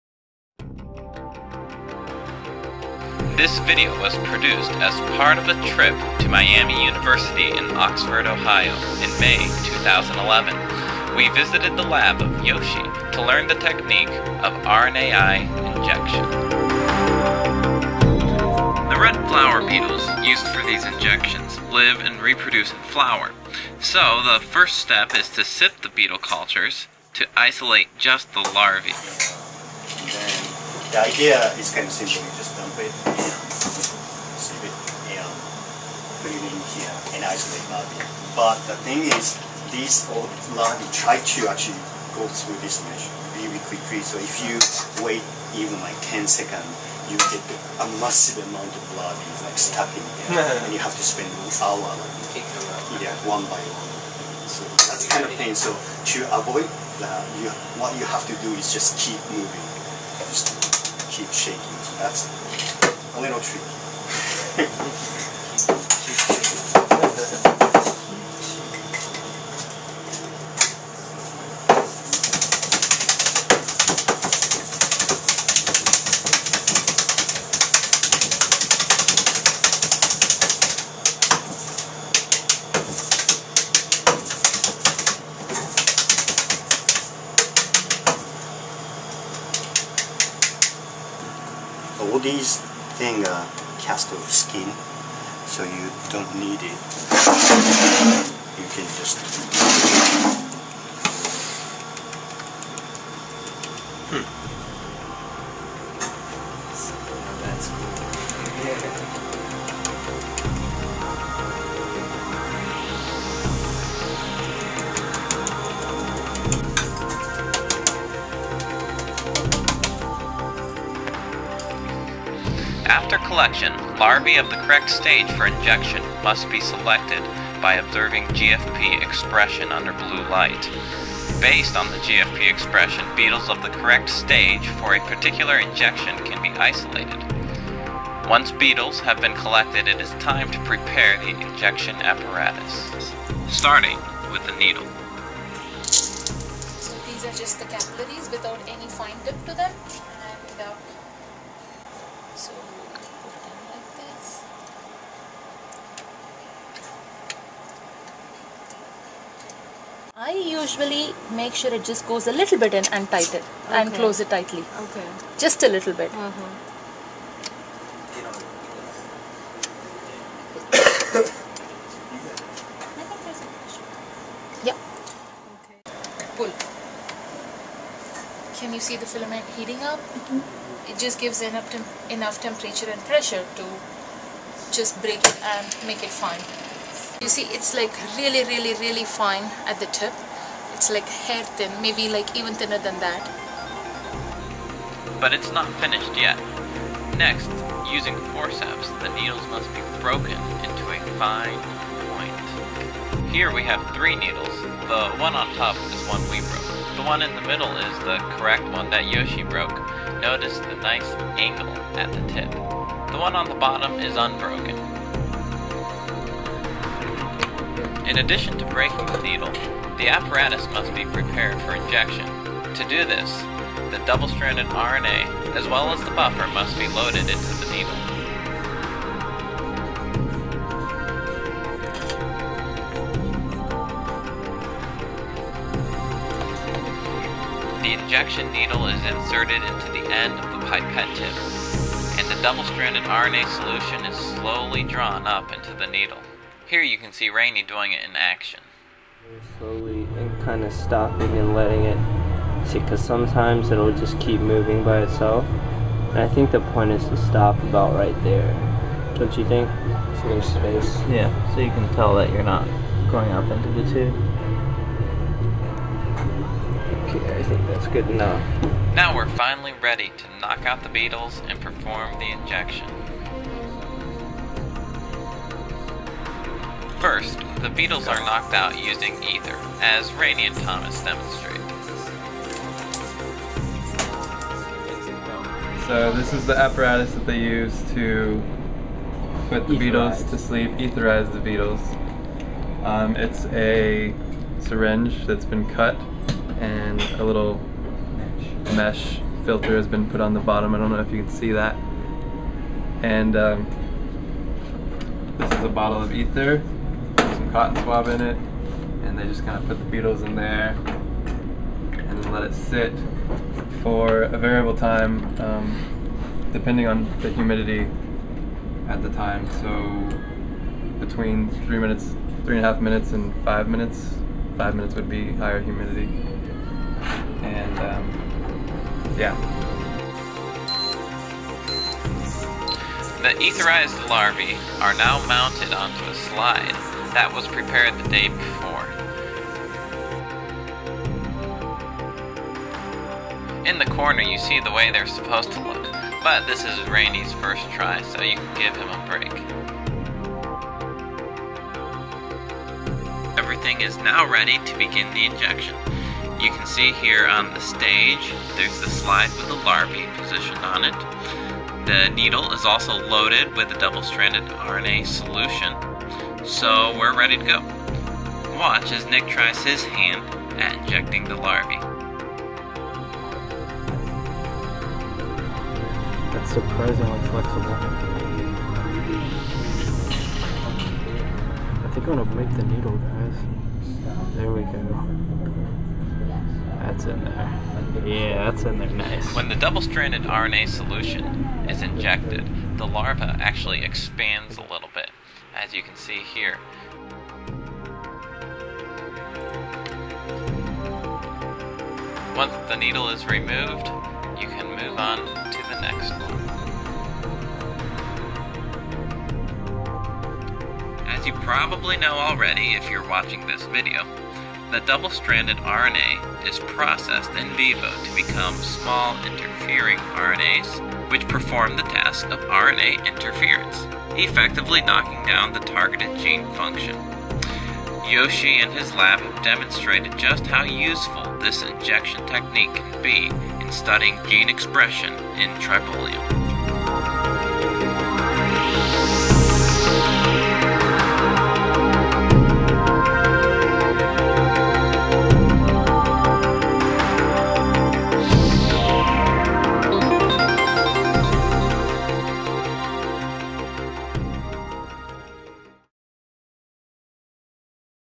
Beetle Injection Technique
Injection of double stranded RNA in beetle larvae can be used to induce targeted systemic RNAi. This video provides an overview of the beetle injection technique used by Dr. Yoshinori Tomoyasu and his beetle lab at Miami University in Oxford, Ohio.